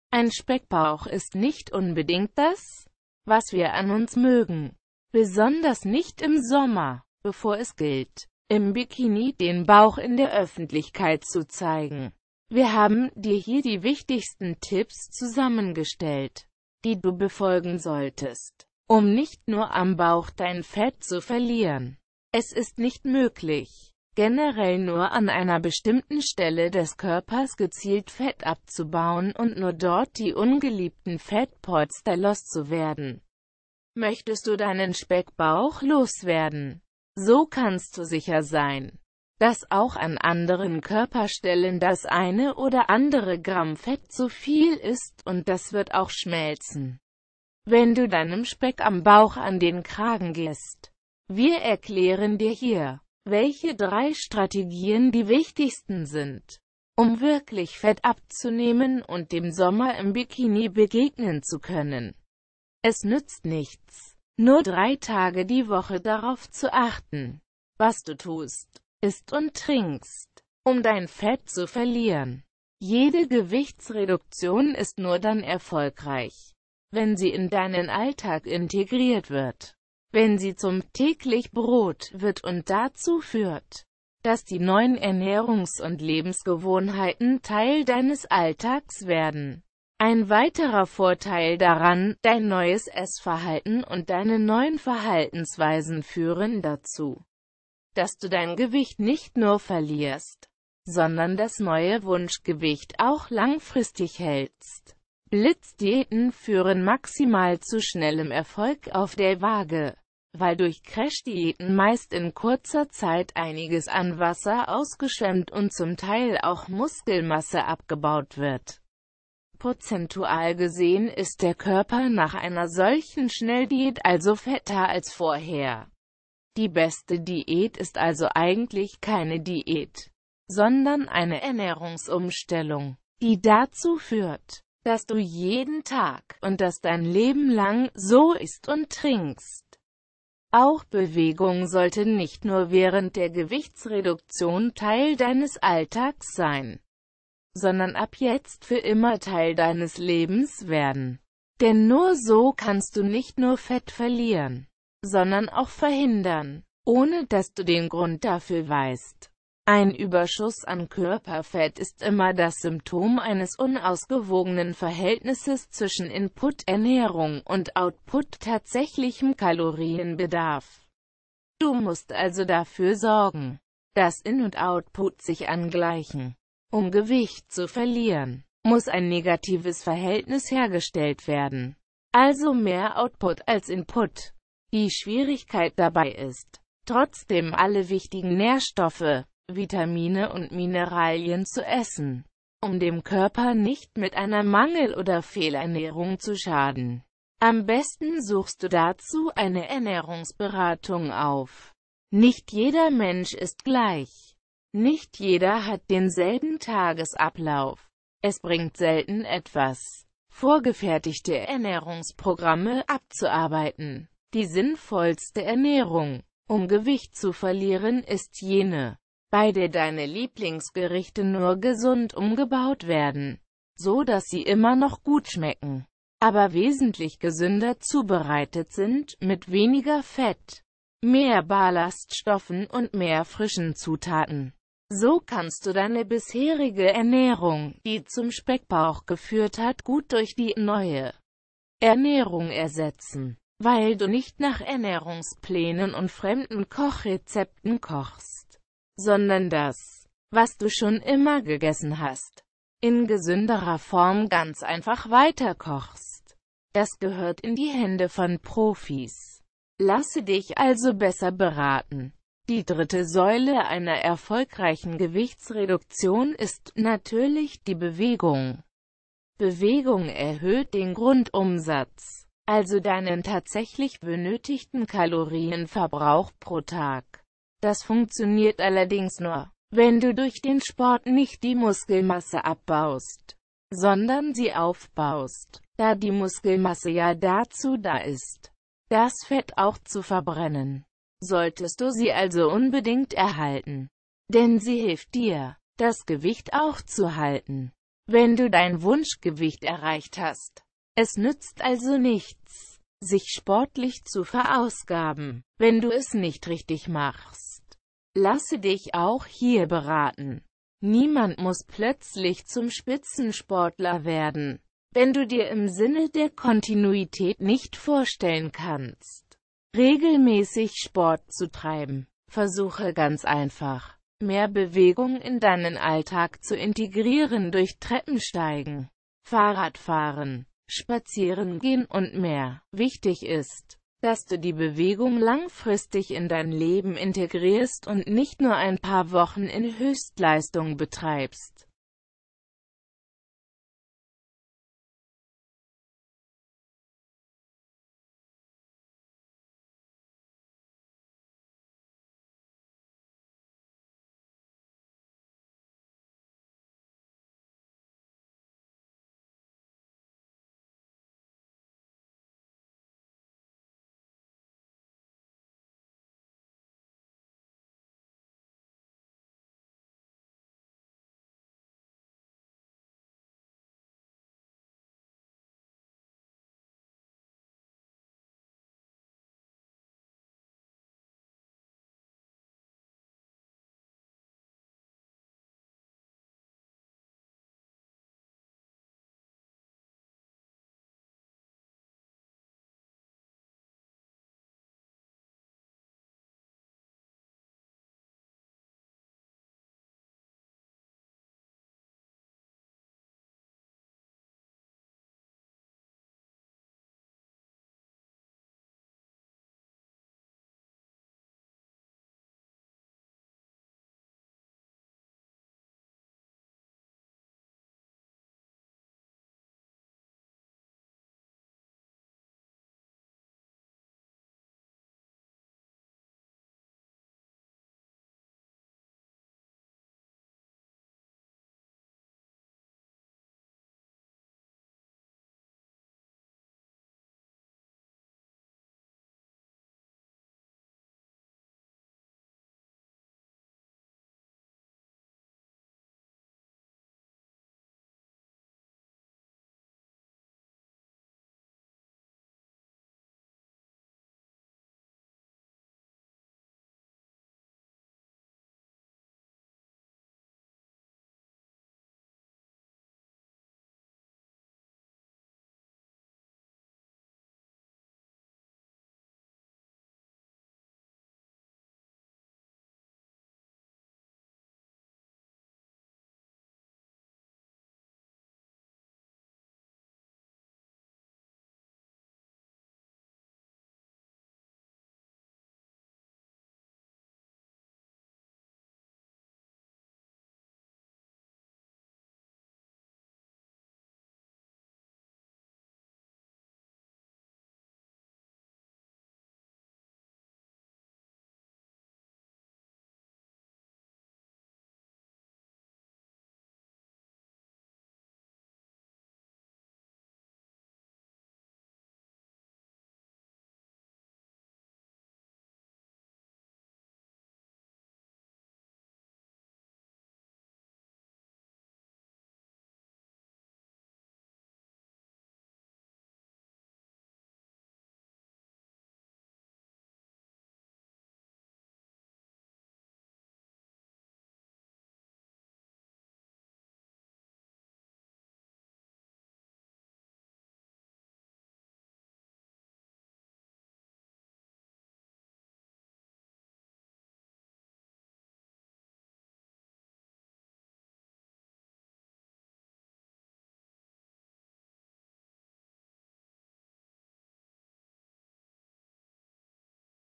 3 wichtige Tipps, um Bauchfett zu verlieren
3 wichtige Tipps, um Bauchfett zu verlieren
Thanks you verry much, like and sub video channel
Ein Speckbauch ist nicht unbedingt das, was wir an uns mögen. Besonders nicht im Sommer, bevor es gilt, im Bikini den Bauch in der Öffentlichkeit zu zeigen. Wir haben dir hier die wichtigsten Tipps zusammen gestellt, die du befolgen solltest, um nicht nur am Bauch dein Fett zu verlieren. Es ist nicht möglich, generell nur an einer bestimmten Stelle des Körpers gezielt Fett abzubauen und nur dort die ungeliebten Fettpolster los zu werden. Möchtest du deinen Speckbauch los werden, so kannst du sicher sein, dass auch an anderen Körperstellen das eine oder andere Gramm Fett zu viel ist – und das wird auch schmelzen, wenn du deinem Speck am Bauch an den Kragen gehst. Wir erklären dir hier, welche drei Strategien die wichtigsten sind, um wirklich Fett abzunehmen und dem Sommer im Bikini begegnen zu können Es nützt nichts, nur drei Tage die Woc